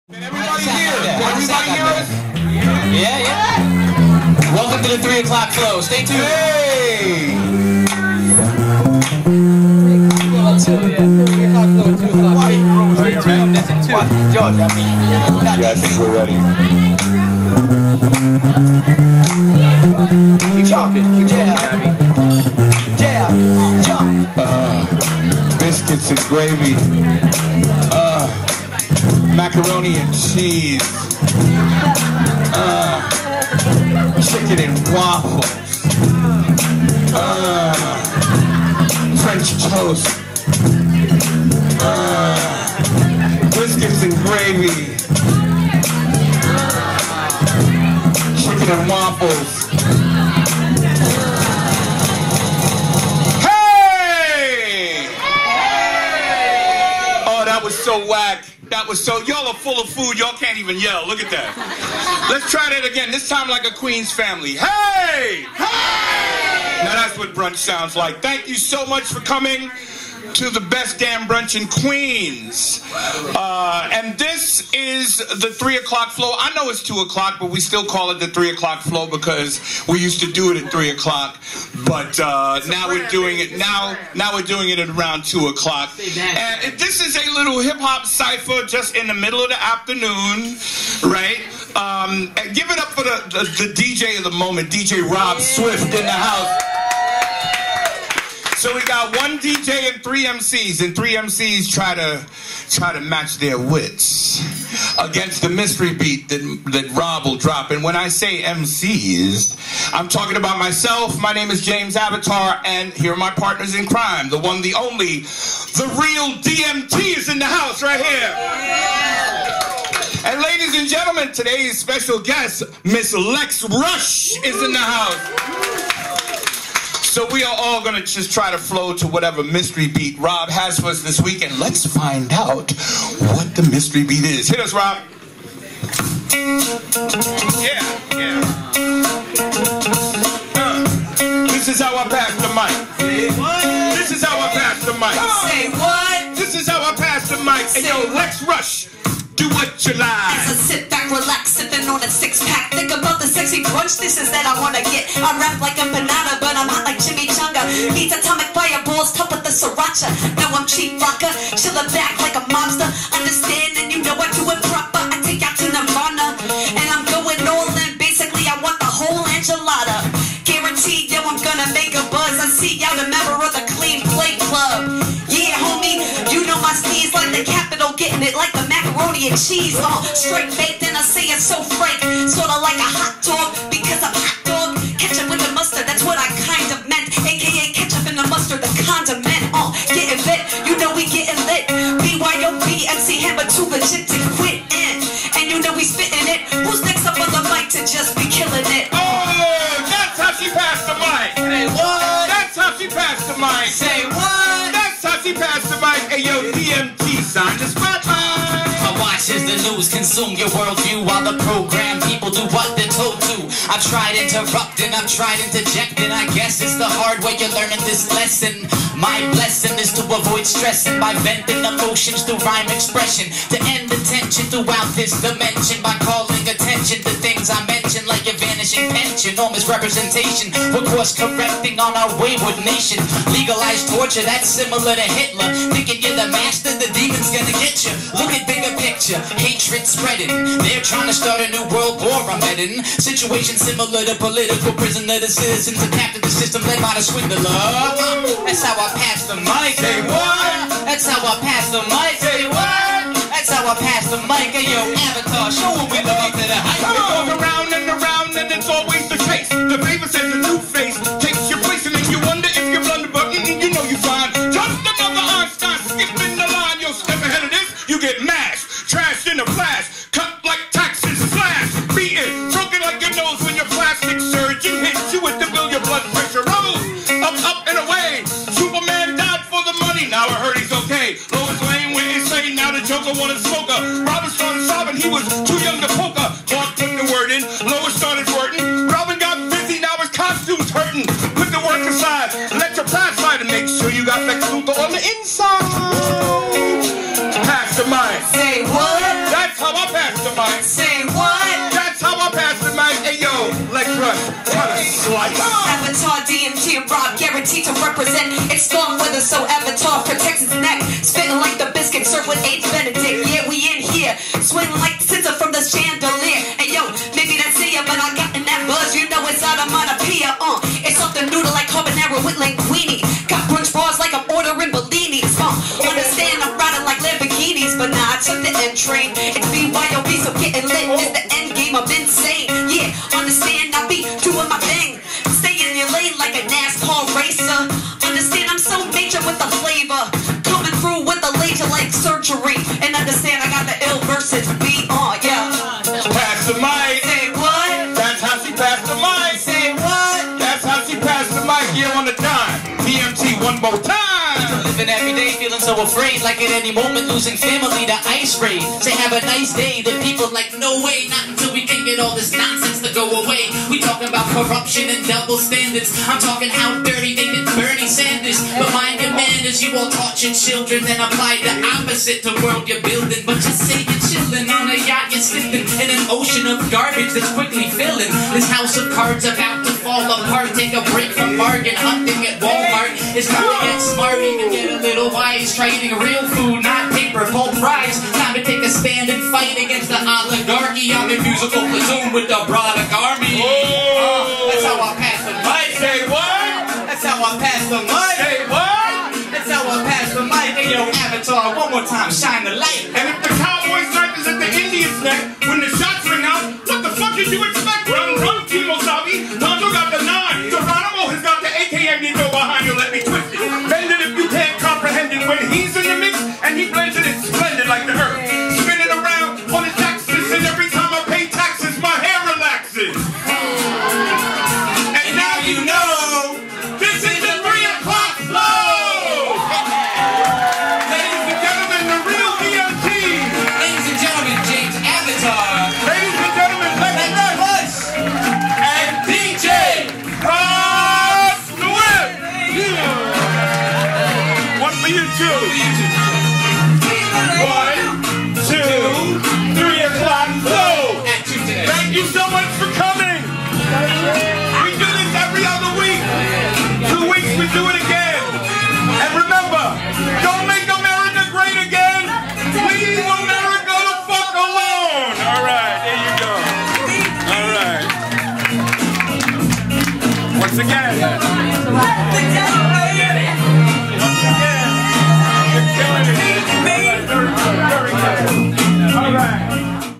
The 3 O'clock Flow at Queens Comfort 12/15/18
James Avatar, The Real DMT, Lex Rush and I are back for another installment of the 3oclockflow.